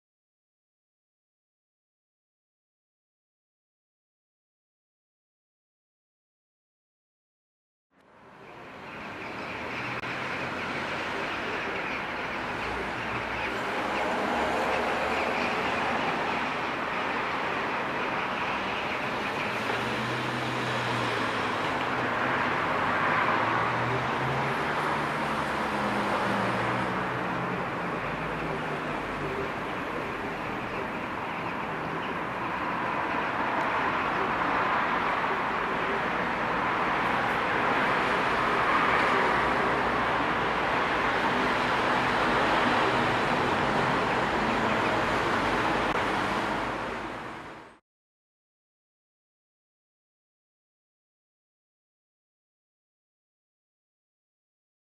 KAJA. Western Jackdaw (Corvus monedula) Klipp - 511
Kaja.  Höstflockar i Västerås Västmanland 3-Oktober 1998, Common Redpoll in Sweden,  AROS FILM Fotograf: Gunnar Fernqvist.